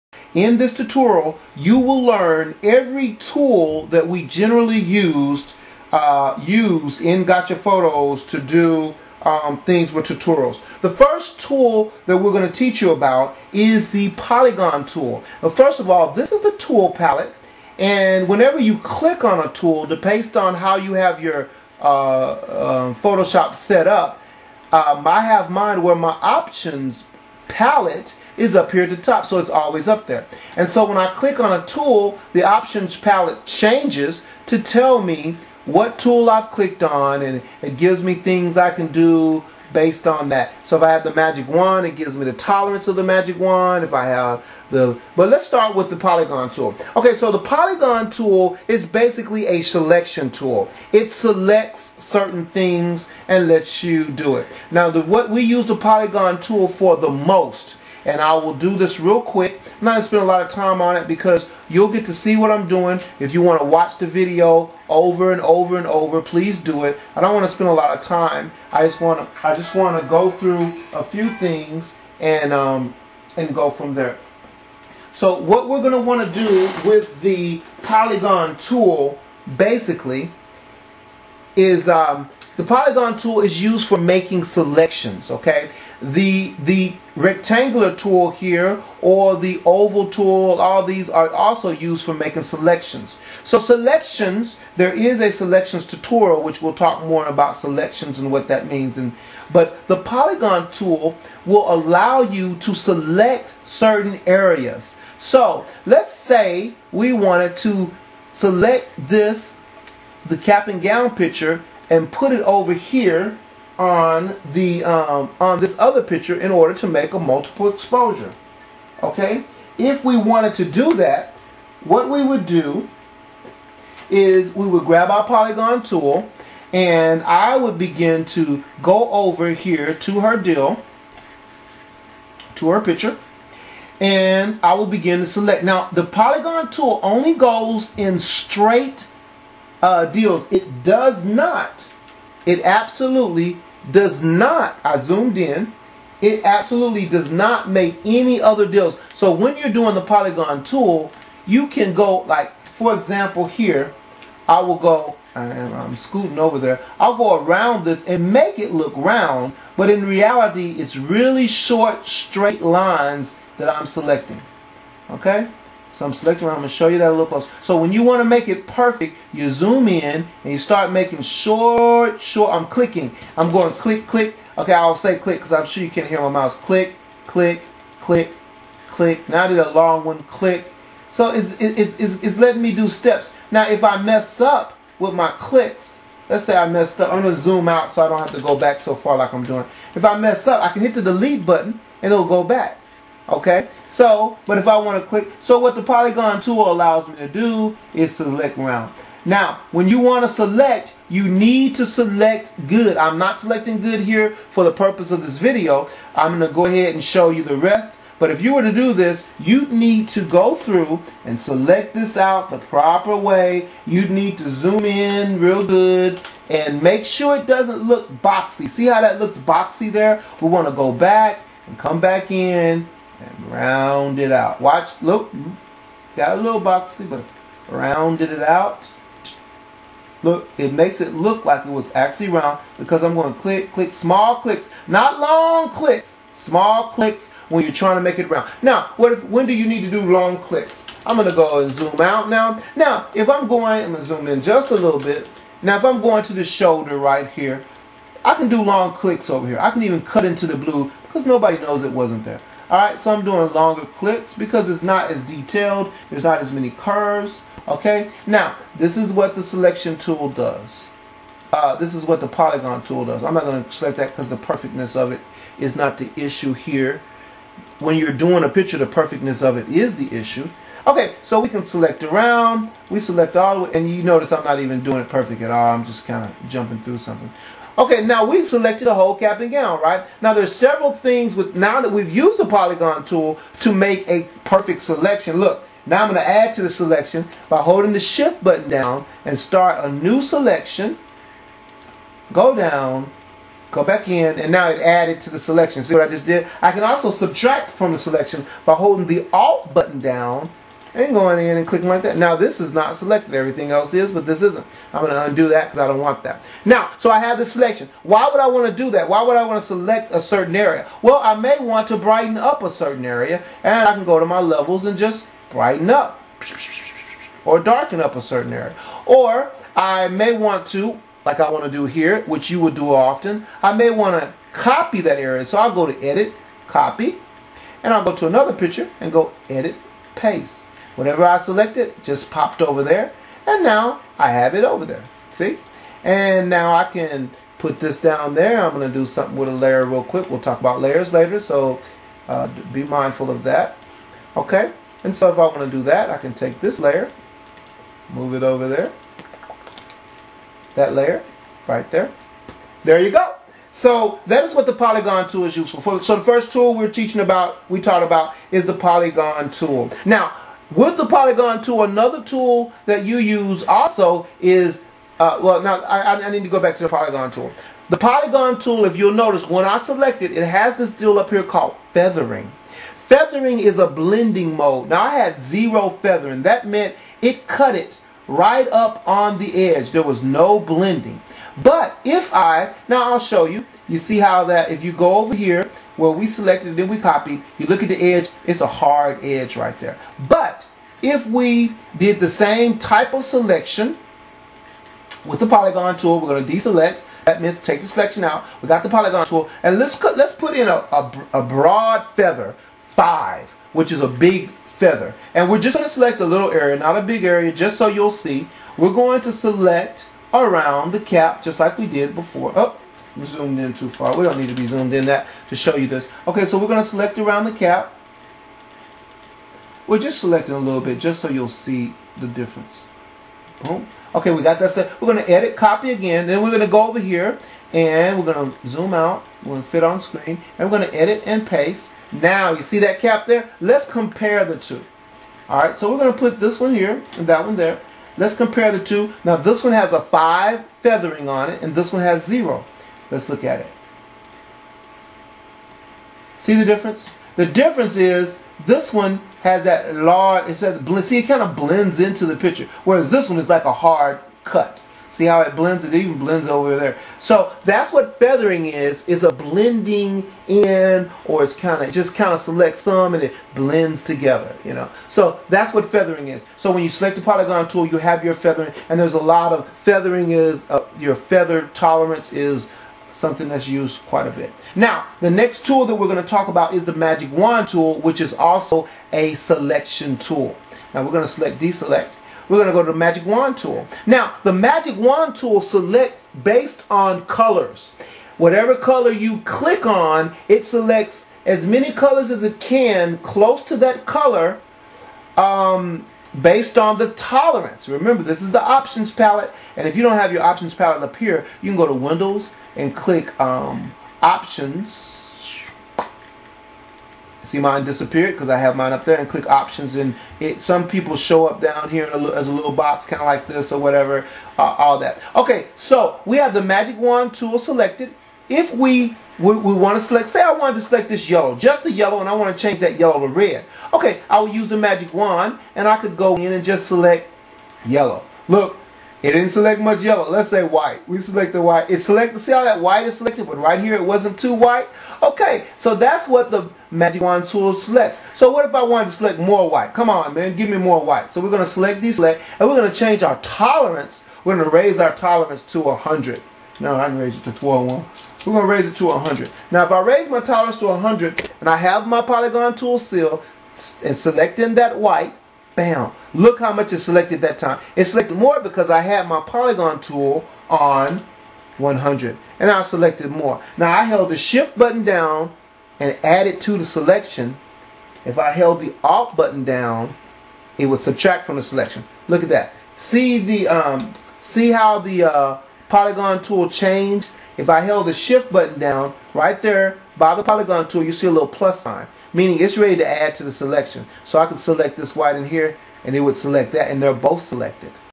Gotcha Photos Tools Tutorial pt 1
Gotcha Photos photo editor's photoshop tutorial that teaches you how to use the most common tools in Photoshop.